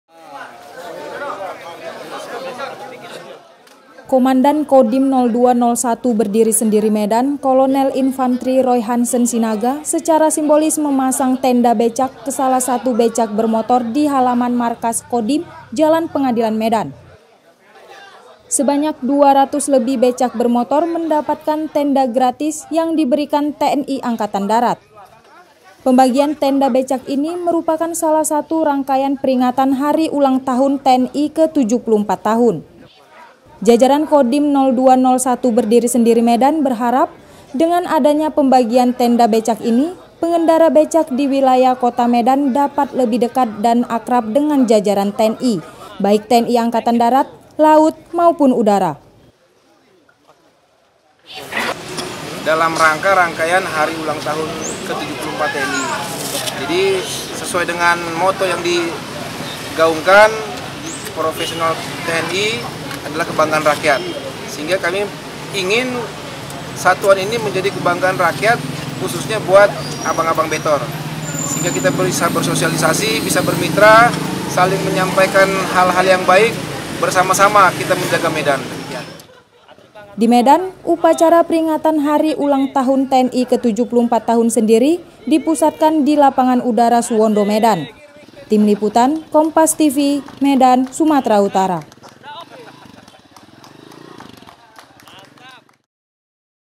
BERBAGI DI HUT TNI
JAJARAN KOMANDO DAERAH MILITER (KODIM) 0201 BERDIRI SENDIRI MEDAN BAGIKAN RATUSAN TENDA UNTUK ABANG BECAK. RANGKAIAN KEGIATAN HUT TNI INI DILAKUKAN UNTUK MENJALIN KEBERSAMAAN TNI DAN MASYARAKAT.